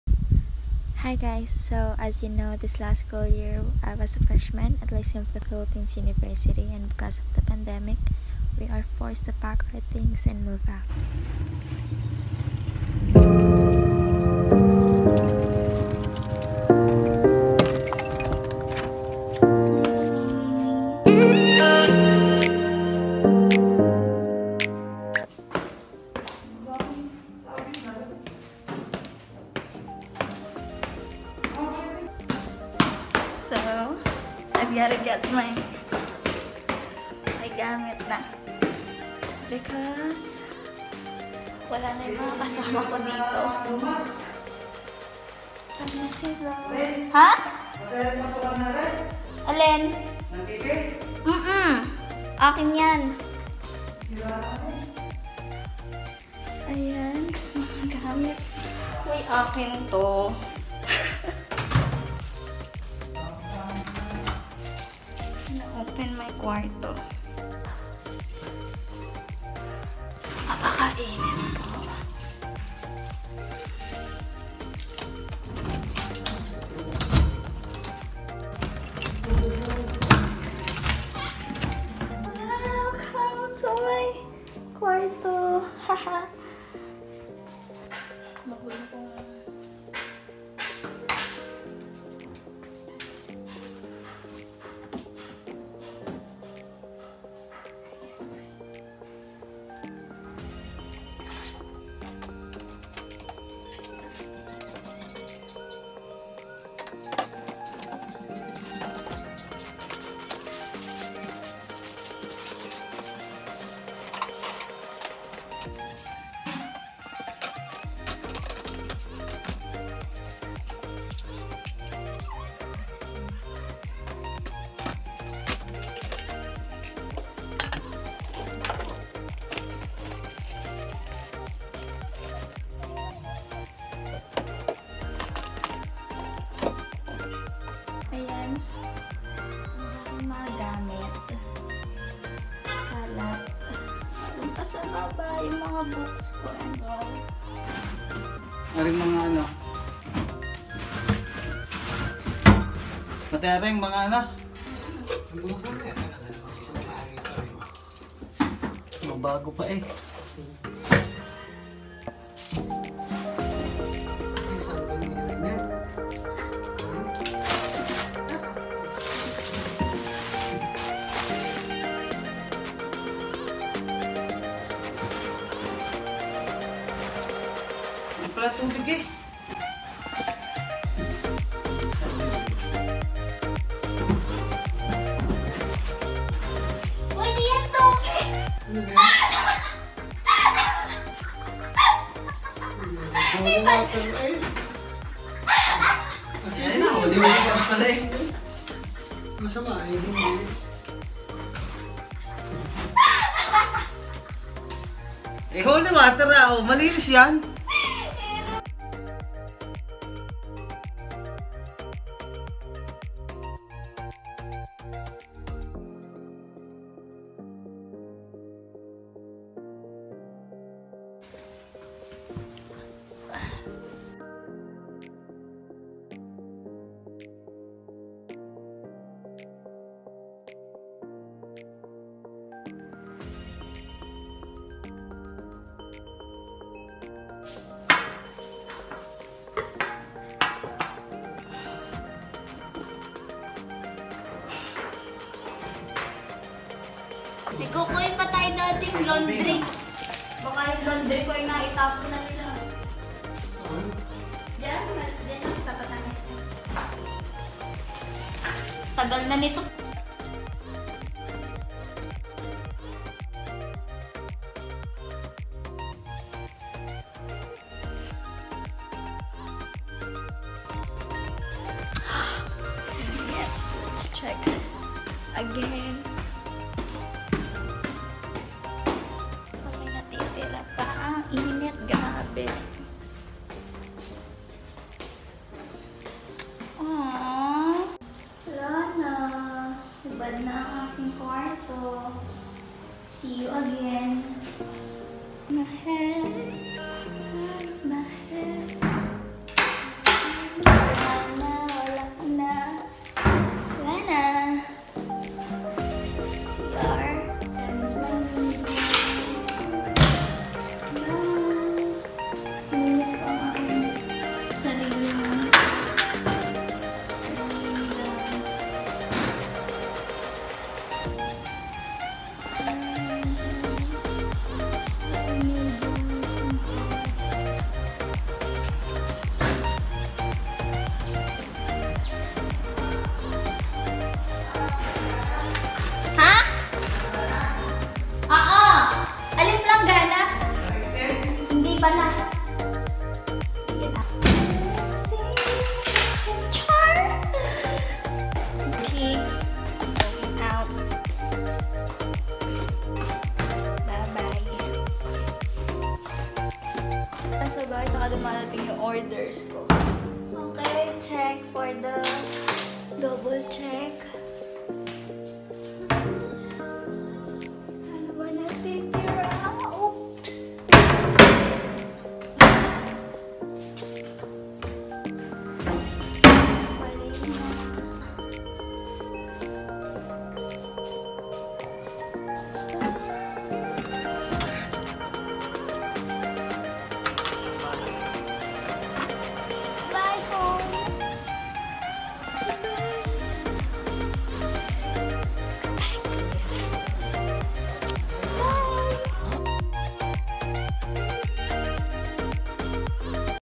COLLEGE MOVE OUT VLOG 2020 | Anne Austria
hi guys hope u all well! so in this video I take you with me as I move out of the college apartment that we lived in for a year, basically this is our first apartment but because of the current situation we need to say goodbye for now. hope u enjoy! love lots!